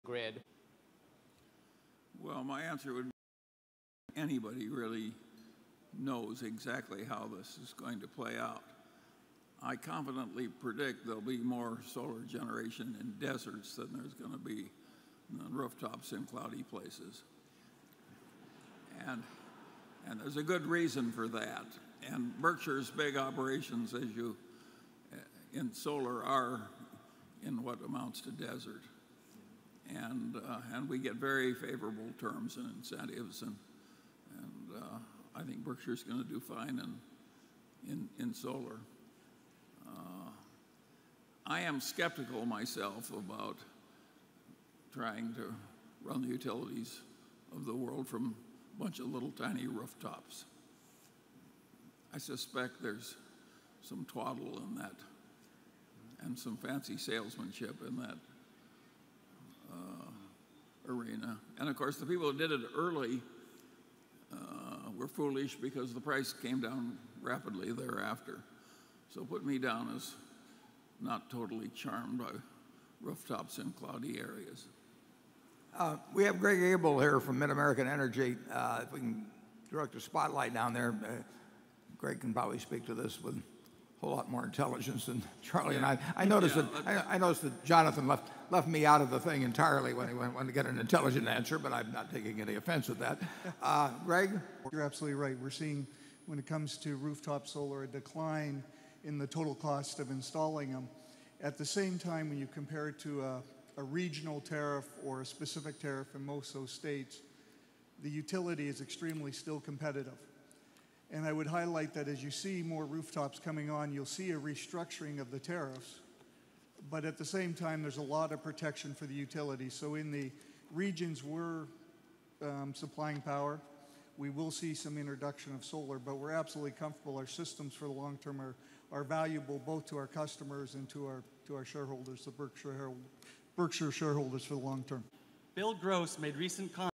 Warren Buffett & Charlie Munger: People Opting for Rooftop Solar a Threat to Berkshire?
Warren Buffett, Charlie Munger and Greg Abel talk about whether people opting for more rooftop solar system is a threat to Berkshire's power generation systems.